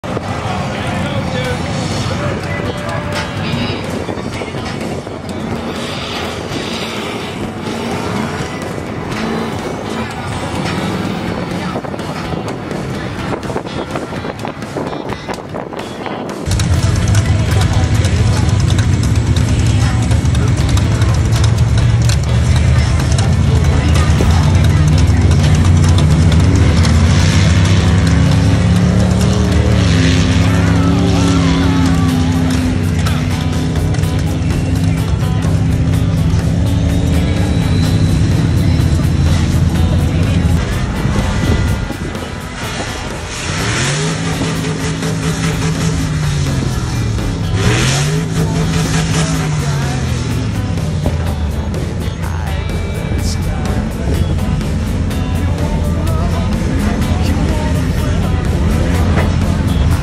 Sink Them or Send Them Trucks
These mud pits are deep and can swallow a truck in a second! It’s not a challenge if you don’t try! This driver didn’t get to far but luckily the excavator is always close by!